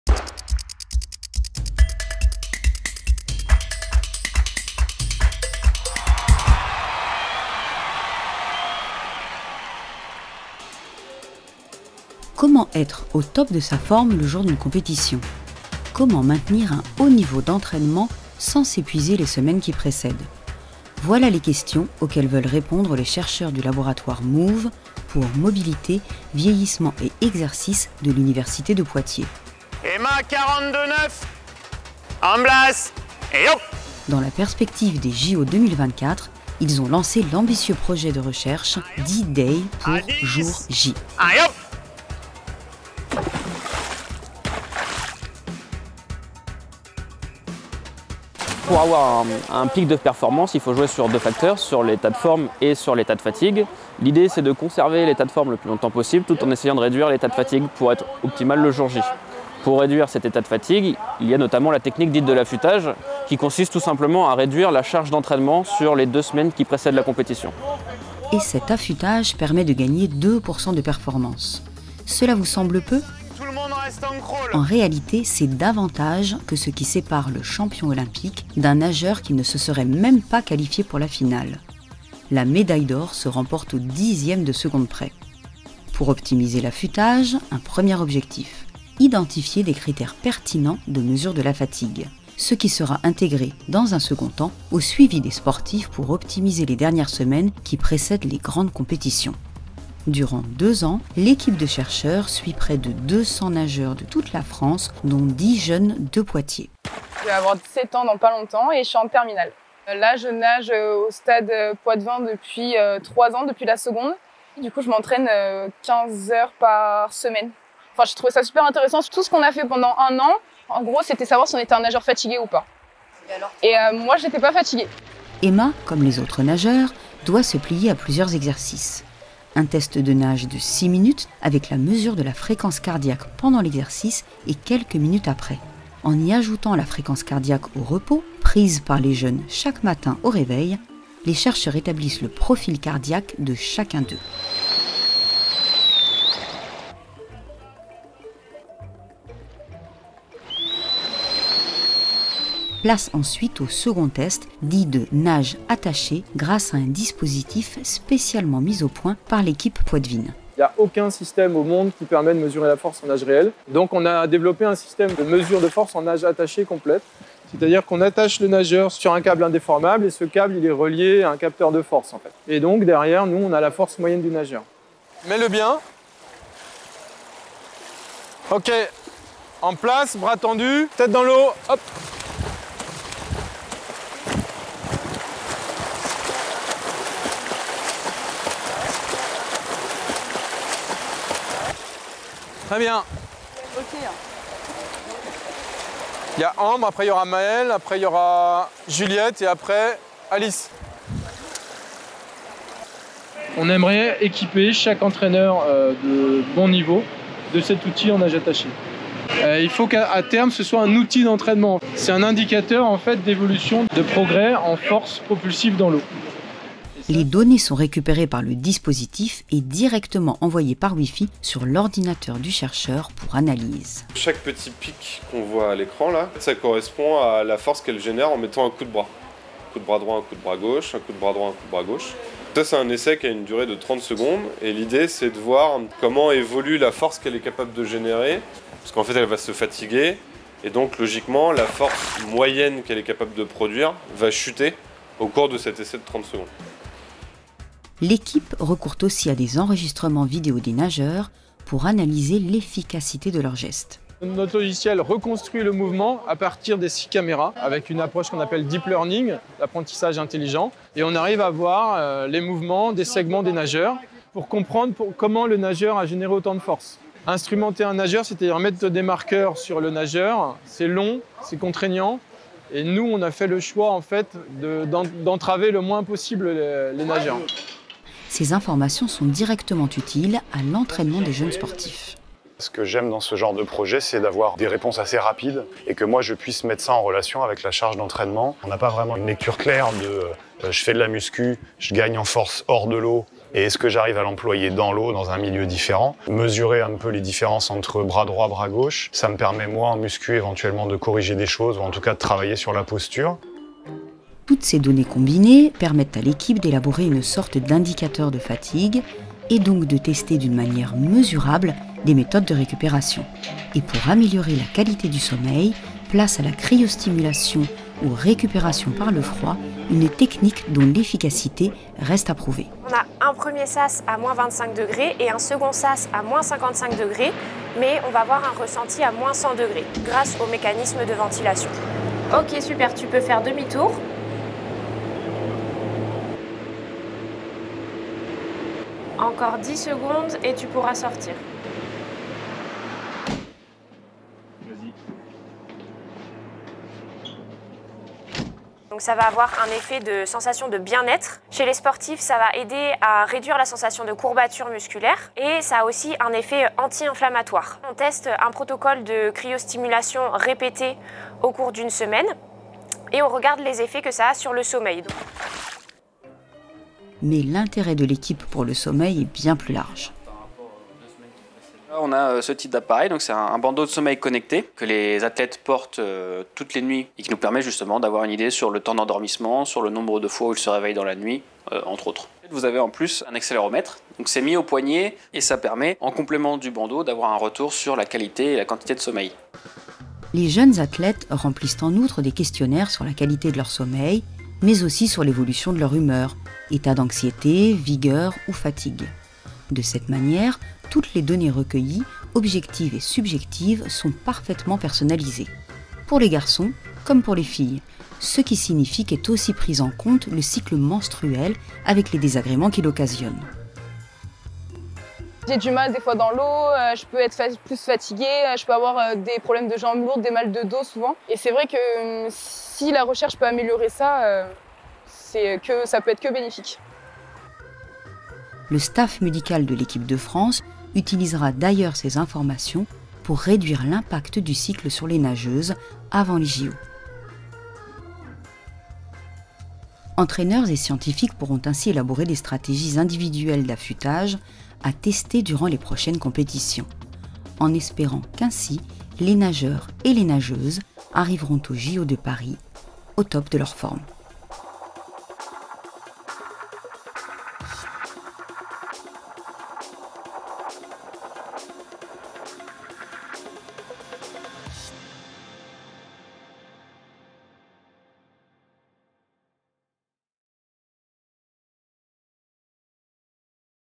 JO 2024 : Comment être entraîné, mais pas fatigué, pour une compétition de natation ? | Reportage
Chercheurs, entraîneurs et sportifs : c’est l’alliance inédite qui s’est nouée pour optimiser les chances des athlètes français aux JO de Paris. Ainsi, en natation, pour remporter une compétition, la moindre fraction de seconde compte. La phase de préparation sportive est donc cruciale : comment s’entraîner intensément sans être fatigué le jour J ? Une question à laquelle une équipe de chercheurs (université de Poitiers, INSEP, CNRS, IRBA) cherche à répondre, grâce au suivi de 200 jeunes athlètes des deux sexes. La science et le sport se rencontrent pour trouver comment améliorer les performances des nageurs.

Journaliste : Barbara Vignaux
JRI, tournage et montage : Marie Brière de la Hosseraye
Production : Universcience 2022